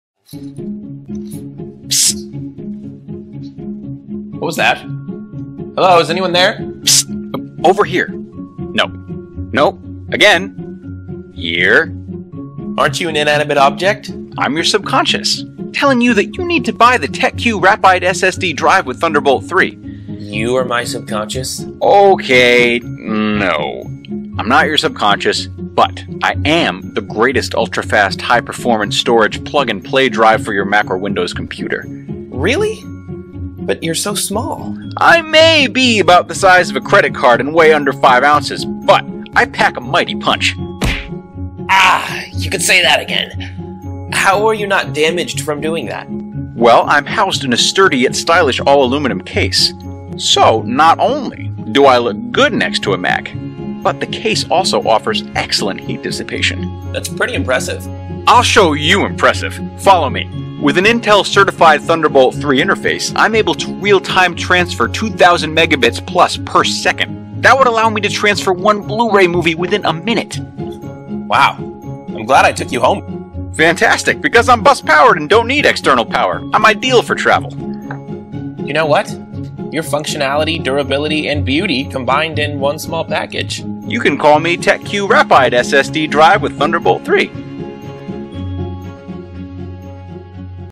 TEKQ Rapide Thunderbolt 3 SSD Portable_Commercial Video
CHECK OUT ON INDIEGOGO & CLAIM YOUR UNIT AT EARLY BIRD PRICE(20%OFF)

ONLY 249 PIECES WOULD BE DELIVERED...DO NOT MISS OUT!!

A Plug-and-Play drive for your Mac & Windows. TEKQ Rapide SSD drive with Thunderbolt 3 delivers ultra-fast, high-performance storage.

Housed in a sturdy yet stylish all-aluminum case, the drive is not only a good match for Mac but it also offers excellent heat dissipation.

Built with Intel Certified Thunderbolt port and SSD at the same level as Kingston KC1000 NVMe PCIe SSD, the drive offers a 2,500MB/s real time transfer speed, allowing you to transfer one Blue-ray movie within 30 seconds.

With our special technique, the drive is Bus-powered and could work perfectly without external AC Power. It weighs only 135g and won't be larger than your palm, so it's very easy to carry your data and the drive everywhere. TEKQ Rapide Thunderbolt 3 SSD  ensures you won’t have to make any compromises when it comes to speed, mobility, and durability.

What is Thunderbolt 3?

Thunderbolt 3 is the latest version of the Thunderbolt interface. Thunderbolt 3 lets you transfer data at up to 40Gbps. That's 2x faster than the 20Gbps speed of Thunderbolt 2, and 4x faster than the 10Gbps speed of USB-C and the original Thunderbolt interface. Thunderbolt 3 lets you connect speedy hard drives, various displays (including 4K and 5K resolutions), and other peripherals, like PCIe Gen 3 expansion cages, to your desktop or laptop.

Highlights
Intel Certified Thunderbolt 3 port

High-speed data transfers

Bus powered, no need external AC power supply

Solid all-aluminum case that's durable and stylish

Excellent heat dissipation

Adopts SSD that is the same level of Kingston KC1000 NVMe PCIe SSD

Real-Time Speed Test:

Tech Specs
Connections: Thunderbolt 3 (USB-C)

Transfer Speed Rate: 40Gb/s

Performance Benchmark : Read 2500MB/s , Write 1500MB/s

Data Storage: 256GB/512GB

Drive Case: Aluminum housing with heat sink embedded for better cooling and heat-dissipating

Power Source: Bus powered

SSD Spec:  NVMe PCIe M.2 SSD with Phison PS5007-E7 Controller

Other Features: Support 2260/2280 form-factor M.2 SSD

LxWxH: 9.8cm x 4.55cm x 1.2cm

Weight: 135g

Device & System Requirements:

PC/Mac with a USB-C (Thunderbolt 3) port
*PC needs to update for the Thunderbolt 3 ports’BIOS, Firmware, and Driver
OS: Mac OS® X 10.10 or later, Windows 8.1 or later